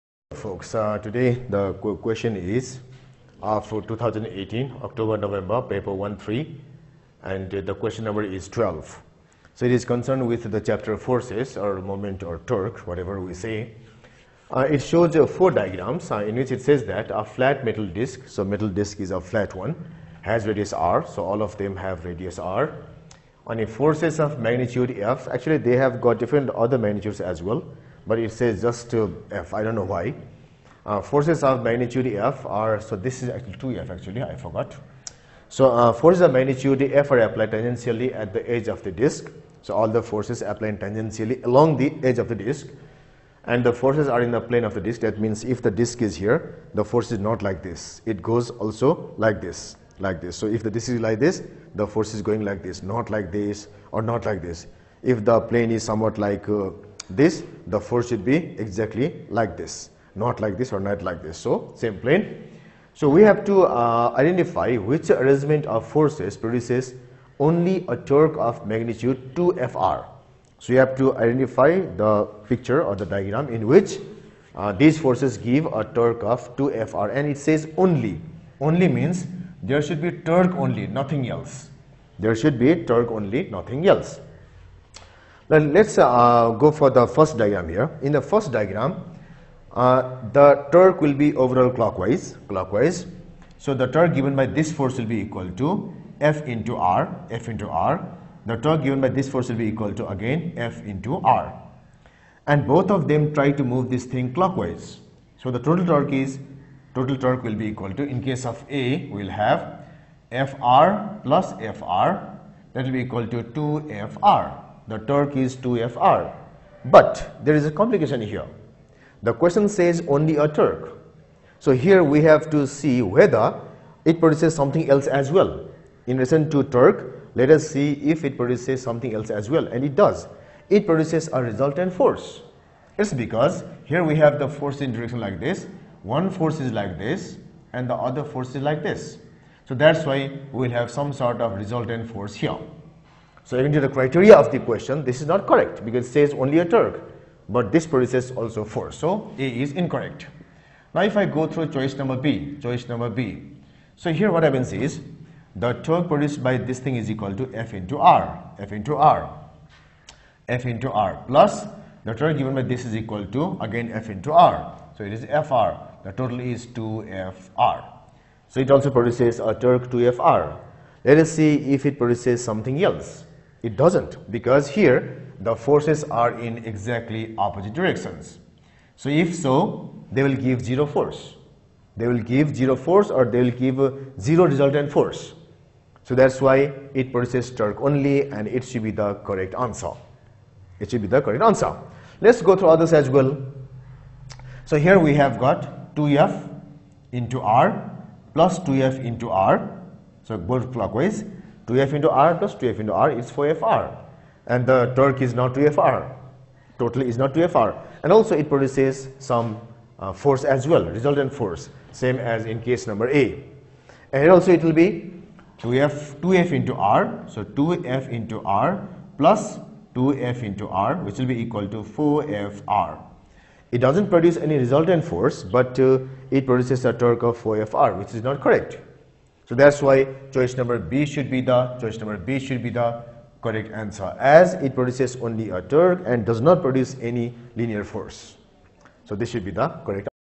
2018 CAIE AS & A level October November Physics Paper 12 Q N 12 (9702/13/O/N/18) by Sajit C Shakya
2018 Cambridge International AS & A level A level October November Physics Paper 12 Q. No. 12 (9702/13/O/N/18) by Sajit Chandra Shakya from Nepal

Chapter - FORCES

A flat metal disc has radius R.

Forces of magnitude F are applied tangentially at the edge of the disc. The forces are in the plane
of the disc.

Which arrangement of forces produces only a torque of magnitude 2FR ?